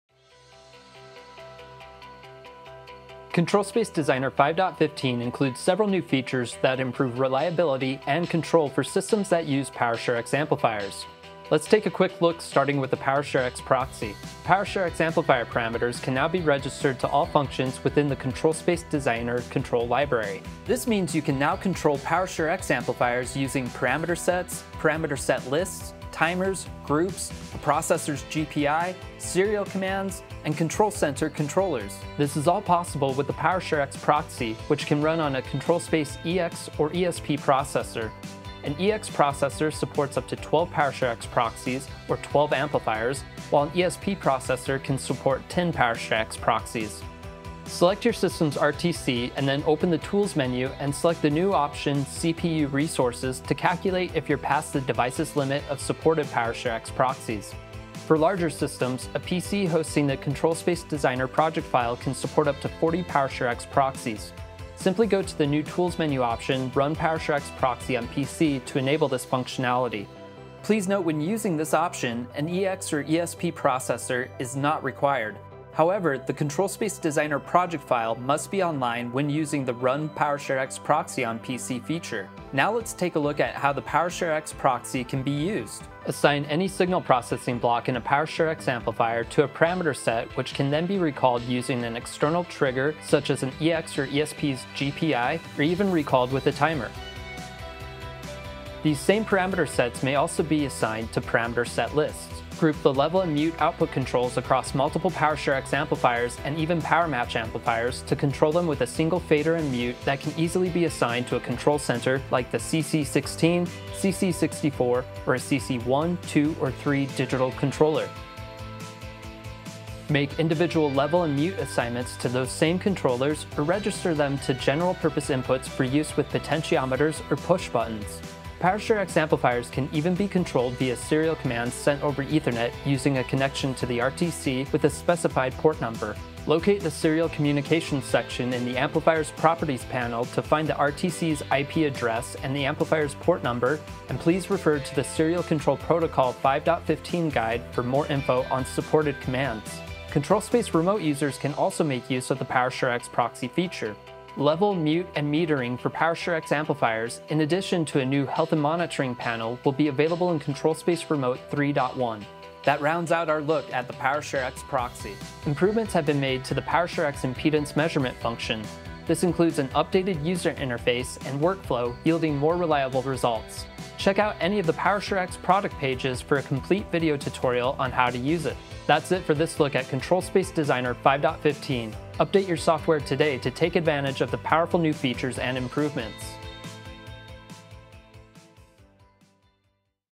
What's New with ControlSpace Designer 5.15
Now Available: ControlSpace Designer 5.15. Take your PowerShareX systems to the next level with new tools that boost control, flexibility, and reliability.
‣ Integrate PowerShareX amps with the full Control Library
‣ Trigger Parameter Sets from GPIs, ControlCenters, or Timers
‣ Group controls across multiple amps—including PowerMatch
‣ Run up to 40 PSXProxies using your PC
‣ New Serial Control options and improved impedance workflow
‣ [Coming soon!] Enhanced integration with ControlSpace Remote 3.1